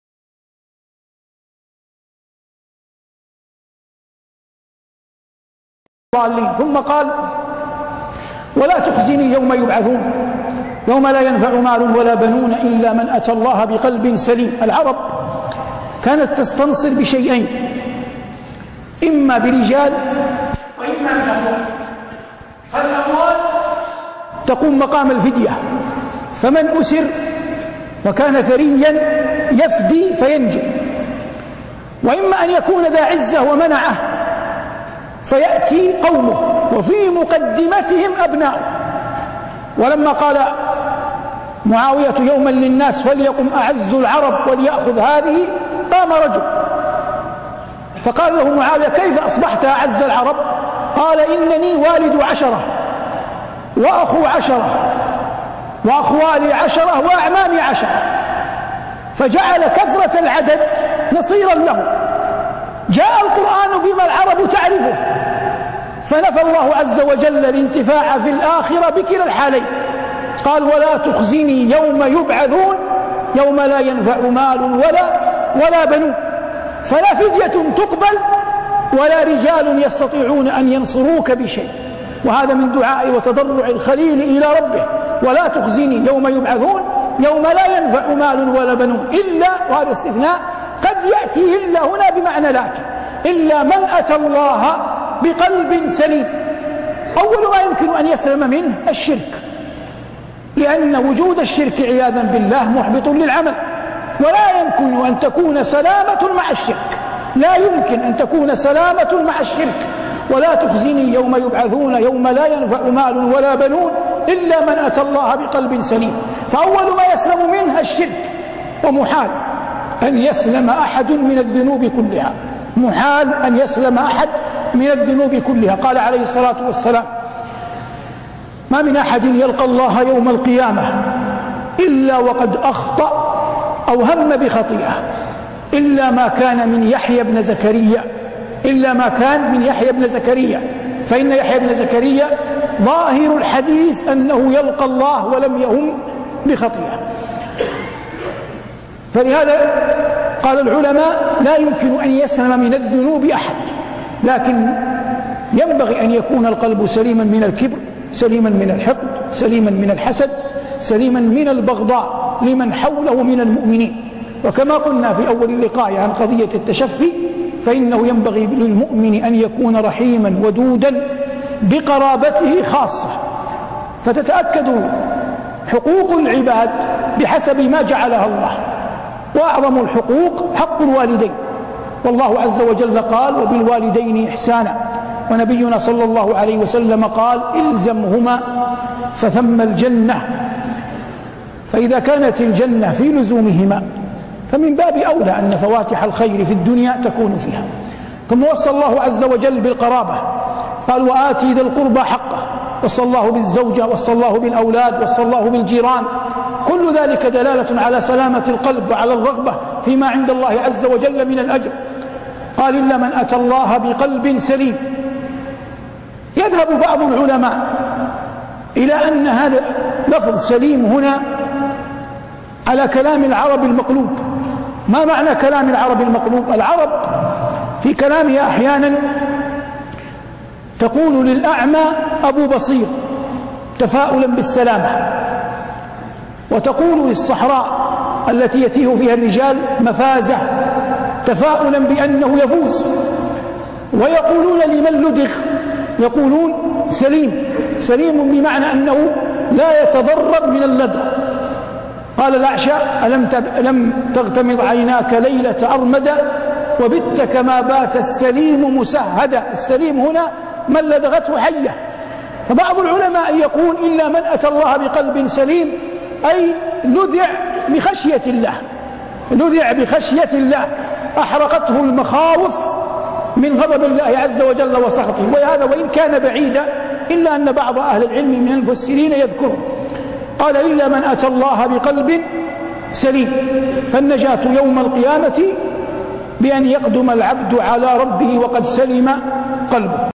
لا نجاة يوم القيامة إلا بسلامة القلب - الشيخ صالح المغامسي
محاضرة بعنوان: "إلا من أتى الله بقلب سليم" بجامع الملك عبد الله بمحافظة الأفلاج